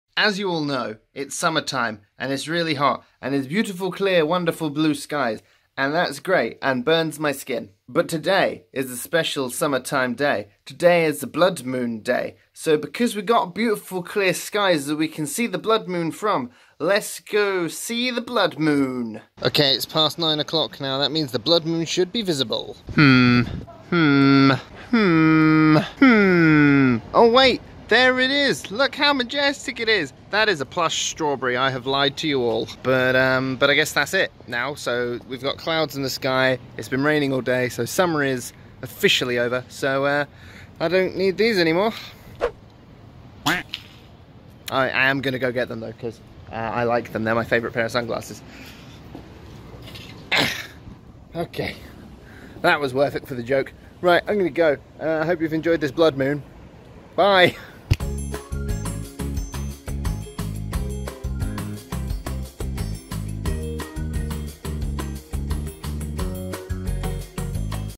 BLOOD MOON IN THE UK?
BloodMoon BloodMoonUK The Blood Moon is in the UK... Somewhere... Not sure where because it turned up on literally the only cloudy day we've had here for like a month! Ahh well, at least I got to wear sunglasses at night!

Filmed entirely on Samsung Galaxy S9!

Stabiliser used: Zhuyun Smooth Q.

Fun British vlogging!